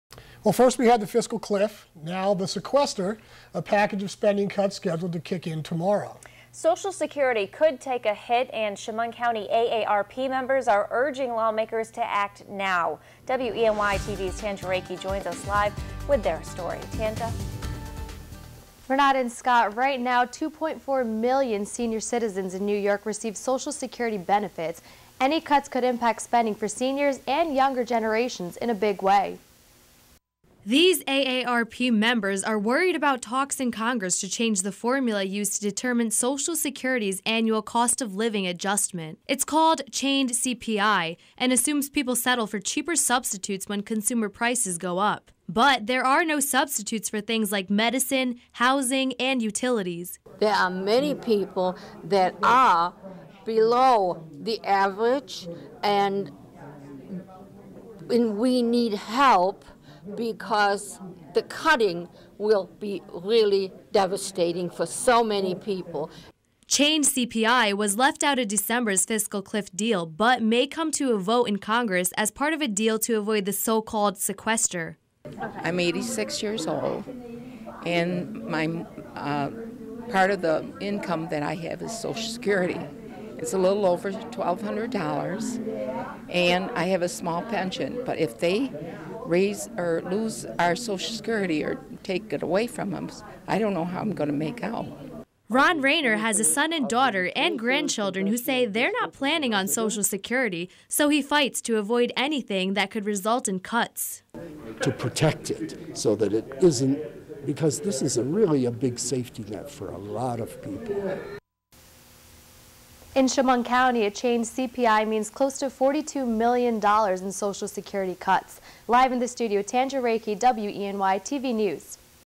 AARP Worries About Sequestration
Social Security could take a hit and Chemung County AARP members are urging lawmakers act now. WENY-TV's Tanja Rekhi reports.